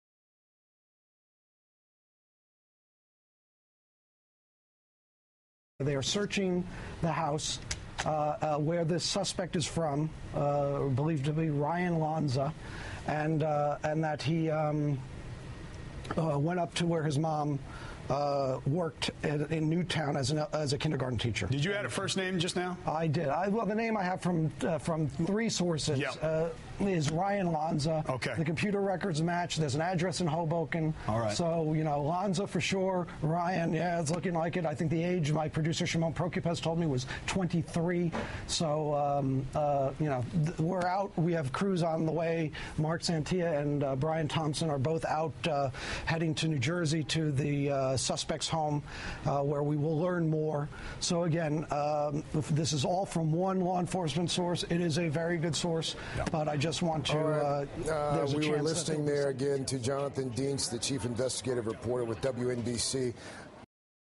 EPIC MEDIA FAIL - RYAN LANZA MISTAKEN as CONNECTICUT SHOOTER
EPIC MEDIA FAIL - RYAN LANZA MISTAKEN as SHOOTER - Wrong Name Publicized